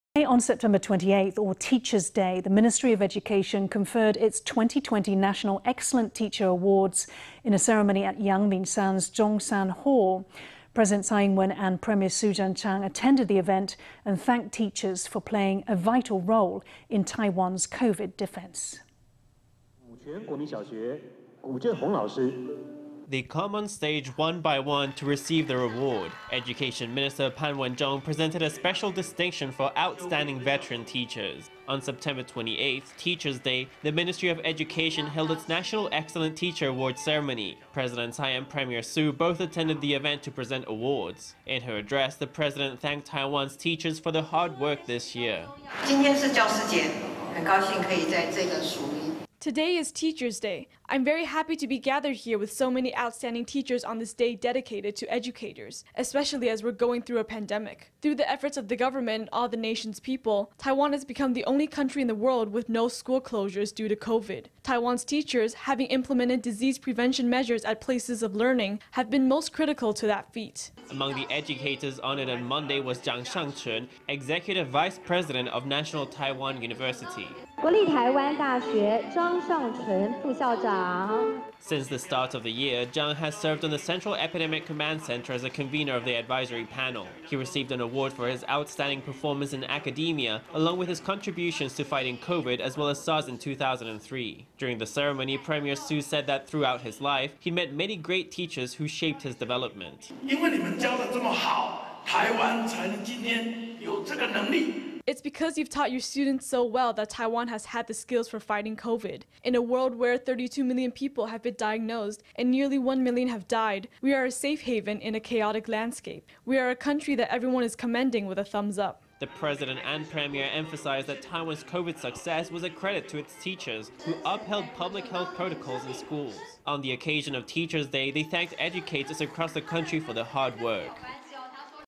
President, premier confer National Excellent Teacher Awards
Today on Sept. 28, or Teachers'' Day, the Ministry of Education conferred its 2020 National Excellent Teacher Awards in a ceremony at Yangmingshan''s Chungshan Hall. President Tsai Ing-wen and Premier Su Tseng-chang attended the event and thanked teachers for playing a vital role in Taiwan''s COVID defense.

They come onstage one by one to receive their award. Education minister Pan Wen-chung presented a special distinction for outstanding veteran teachers.

On Sept. 28, Teacher’s Day, the Ministry of Education held its National Excellent Teacher Award ceremony. President Tsai and Premier Su both attended the event to present awards. In her address, the president thanked Taiwan’s teachers for their hard work this year.

Tsai Ing-wen
President
Today is Teachers'' Day. I’m very happy to be gathered here with so many outstanding teachers on this day dedicated to educators, especially as we’re going through a pandemic. Through the efforts of the government and all the nation’s people, Taiwan has become the only country in the world with no school closures due to COVID. Taiwan’s teachers, having implemented disease prevention measures at places of learning, have been most critical to that feat.

Among the educators honored on Monday was Chang Shan-chwen, executive vice president of National Taiwan University.

Since the start of the year, Chang has served on the Central Epidemic Command Center as convener of the advisory panel. He received an award for his outstanding performance in academia, along with his contributions to fighting COVID as well as SARS in 2003. During the ceremony, Premier Su said that throughout his life, he’d met many great teachers who shaped his development.

Su Tseng-chang
Premier
It’s because you’ve taught your students so well that Taiwan has had the skills for fighting COVID. In a world where 32 million people have been diagnosed and nearly 1 million have died, we are a safe haven in a chaotic landscape. We are a country that everyone is commending with a thumbs-up.

The president and premier emphasized that Taiwan’s COVID success was a credit to its teachers, who upheld public health protocols in schools. On the occasion of Teachers’ Day, they thanked educators across the country for their hard work.